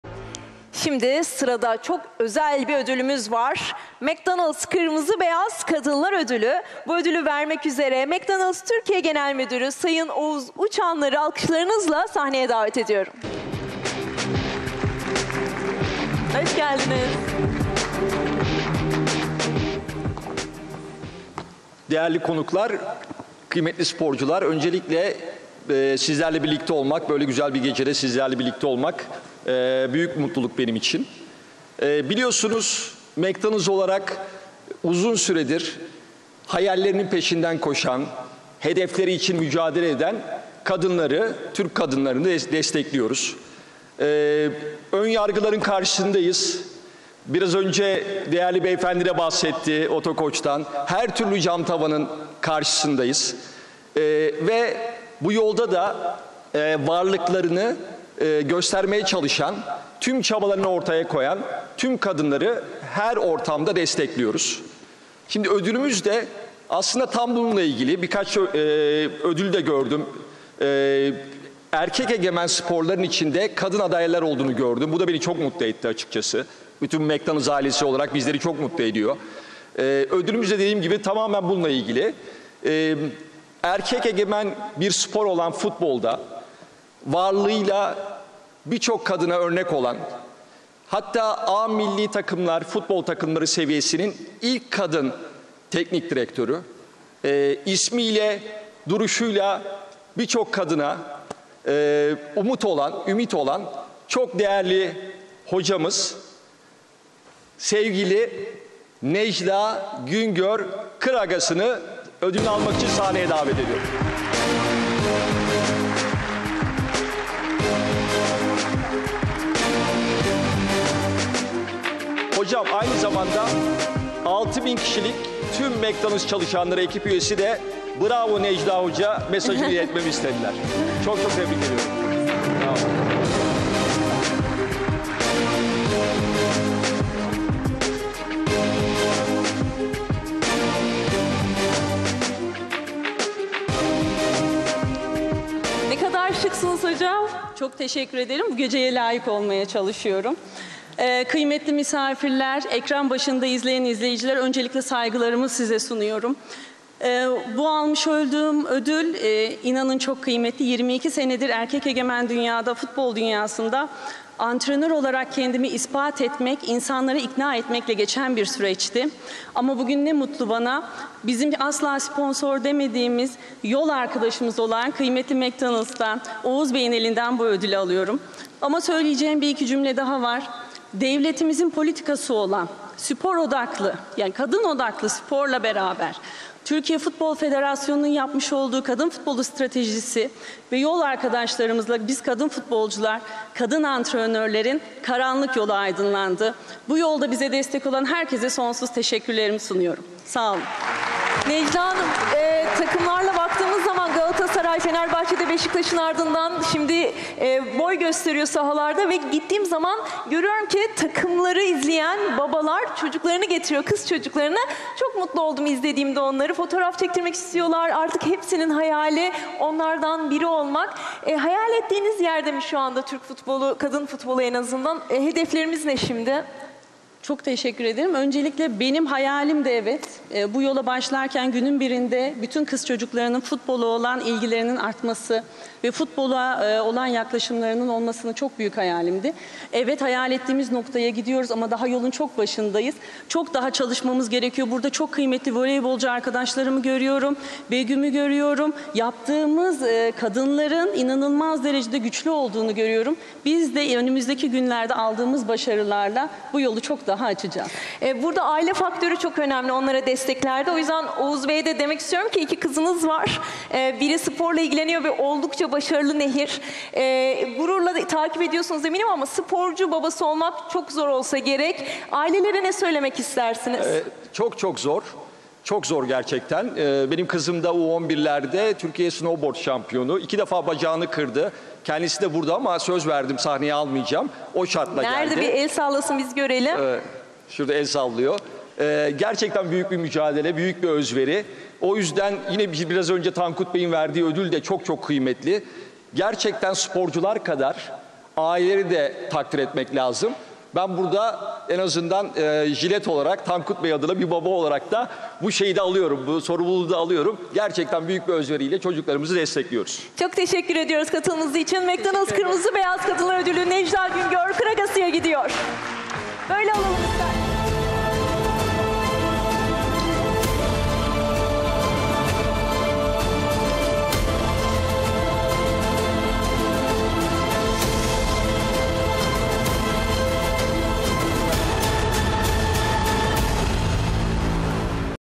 MCDONALD'S KIRMIZI - BEYAZ KADINLAR ÖDÜLÜ | NECLA GÜNGÖR KIRAGASI
68. Gillette Milliyet Yılın Sporcusu Ödülleri’nde "McDonald’s Kırmızı – Beyaz Kadınlar Ödülü" A Milli Kadın Futbol Takımı Teknik Direktörü Necla Güngör Kıragası'nın oldu.

Spora dair birbirinden farklı içerikleri sportstv'nin youtube hesabından takip edebilirsiniz.